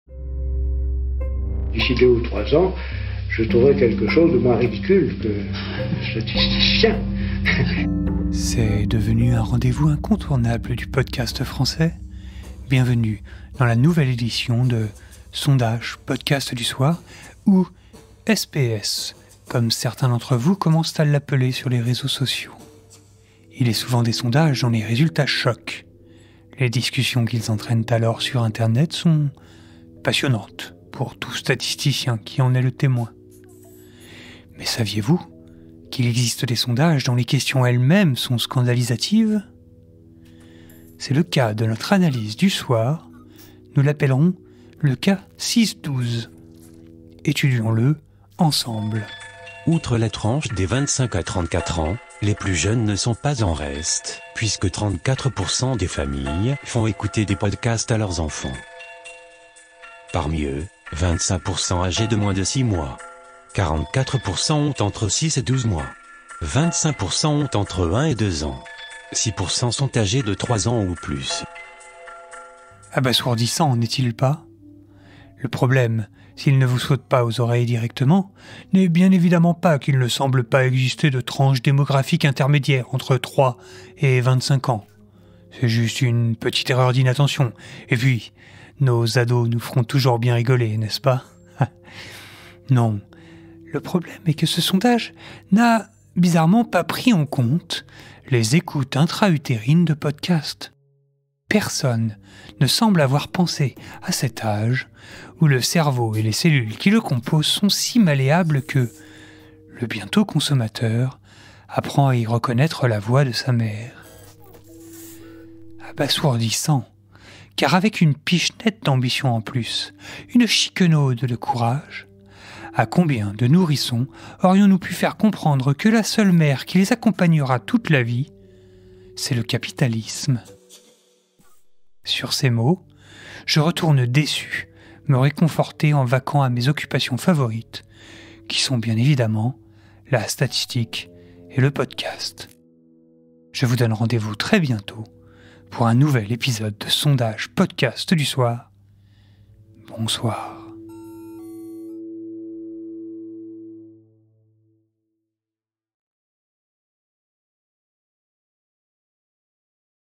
[Podcast] Sondage Podcast du soir - Episode 05 - 6-12
Le PREMIER podcast qui propose une analyse détaillée d'un sondage concernant le monde du podcast, le soir. Proposé et présenté par Thomas JEAN-PATSAROUPTE.

Episode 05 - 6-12 épisode de Sondage Podcast du soir 📻 Analyses détaillées et rigoureuses`

Sondage Podcast du soir une émission qui s'adresse à tous les auditeurs et auditrices de 7 à 77 mois, 77 ans ? 77... je sais plus. Je suis un peu perdu.
Ce soir, nous parlerons justement de ces sondages qui n'apportent rien, ni à leur créateurs, ni à ceux qui les publient, ni à ceux qui les lisent. On dit que ces sondages représenteraient plus de 42% des sondages et que seul 3% de la population s'en aperçoit. Le constat donne la chair de poule.

Rendez-vous samedi prochain pour un nouvel épisode de Sondage Podcast du soir, bonsoir.